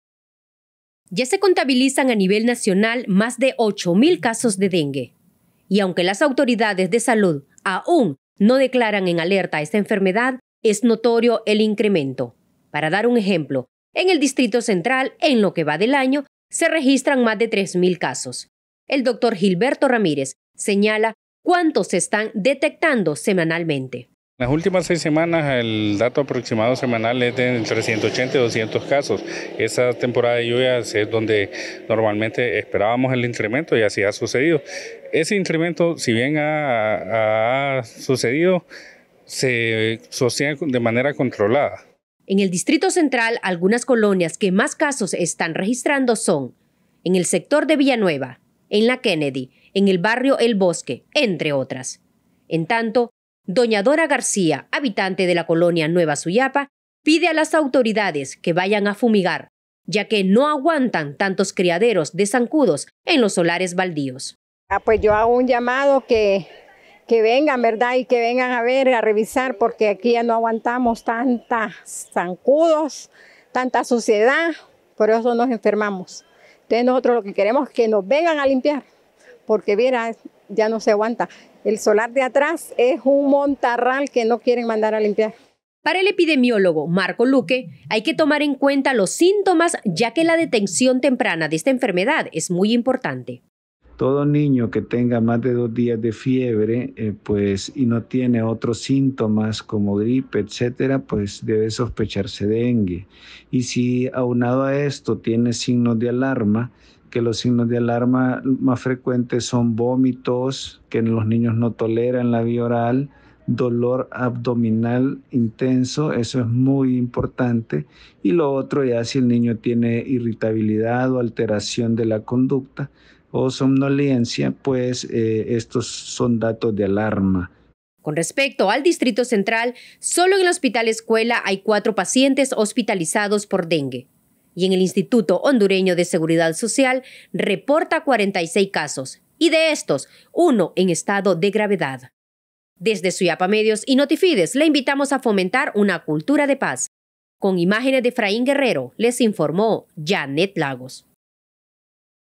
8 mil casos de dengue
A nivel nacional ya se contabilizan más de 8 mil casos de dengue, aunque las autoridades de la Secretaria de Salud, aun no declaran alerta en los departamentos que más se están presentando enfermos con esta patología.